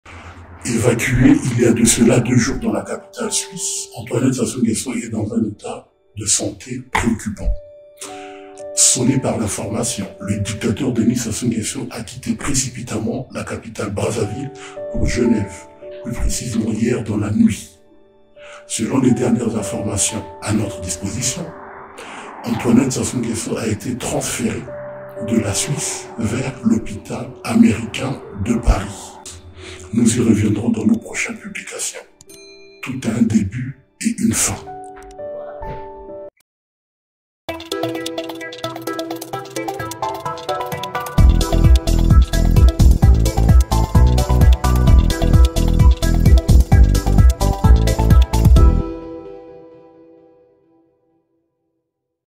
ANTOINETTE SASSOU NGUESSO DANS UN ÉTAT CRITIQUE
La femme du Dictateur Denis Sassou Nguesso serait dans un état critique à l'hôpital américain de Paris.

Zilliqa (ZIL): zil1avll75n5slmq67jgufqhv7f3y6vaa9x8st52gn

DEVENIR MEMBRE

NOUS SUIVRE

NOUS CONTACTER